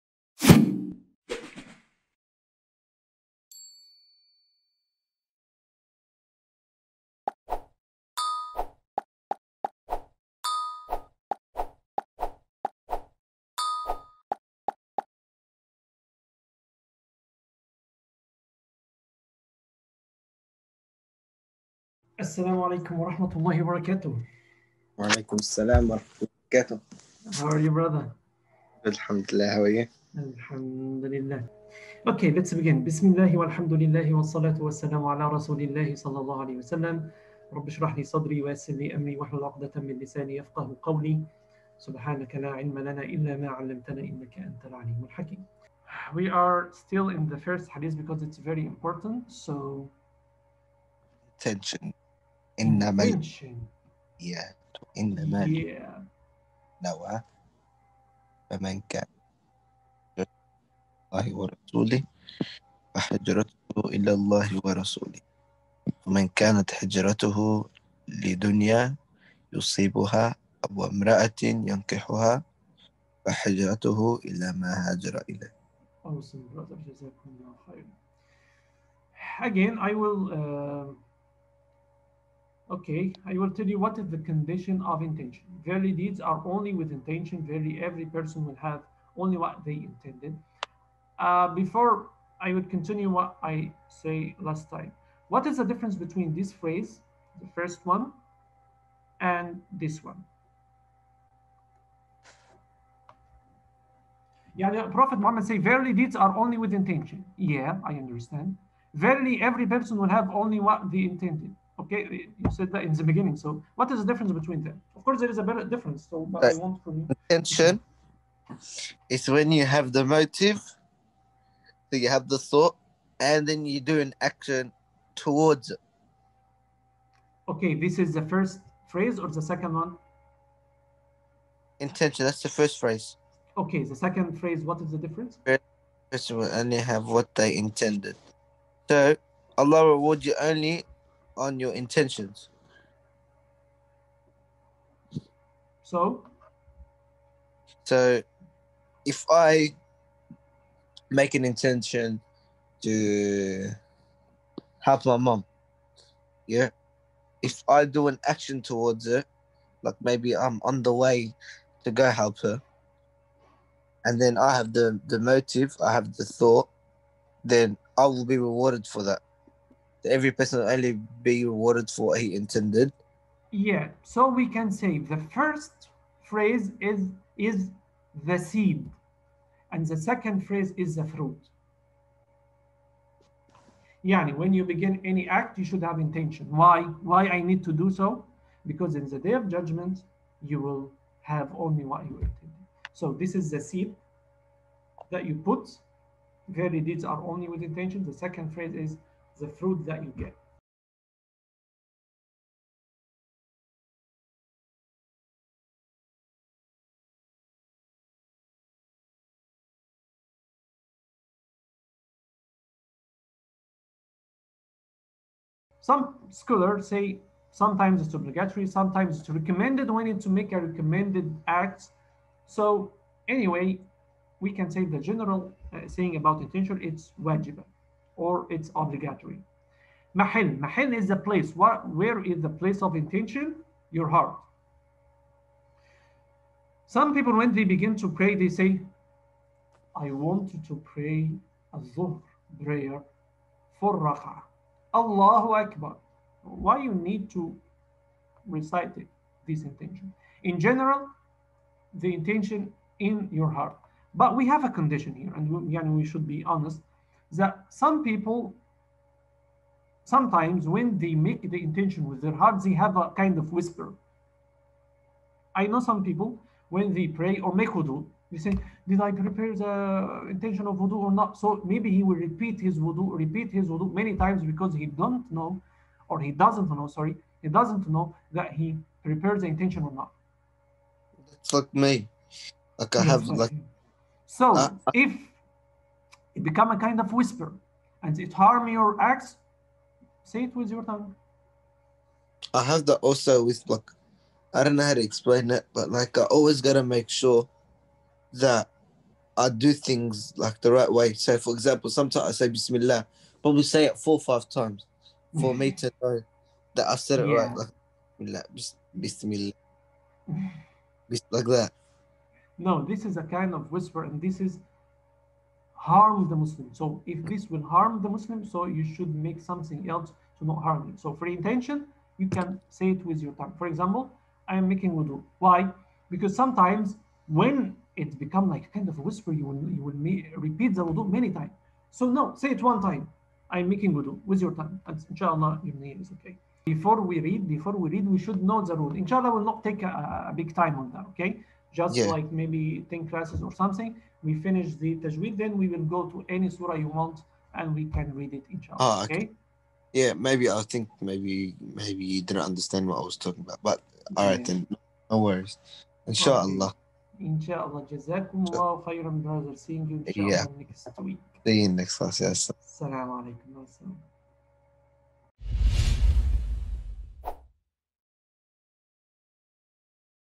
IJAAZAH Lesson: Explanation of Forty Hadith An-Nawawi
Join us in this IJAAZAH lesson as we explore the Forty Hadith of Imam An-Nawawi. Discover the profound wisdom and guidance contained in these essential hadiths.

👉 For more information and to join, visit our